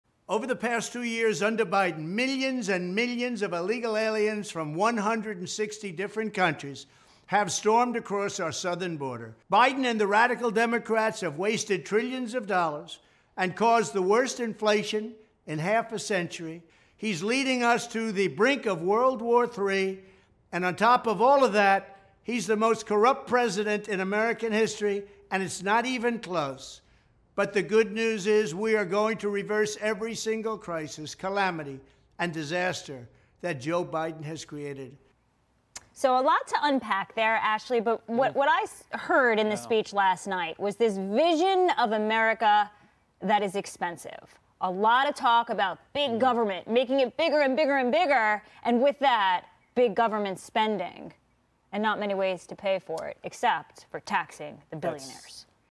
Trump accuses Biden of leading US to ‘brink of World War III’
In a political ad, former President Donald Trump vows to reverse problems cause by the Biden administration.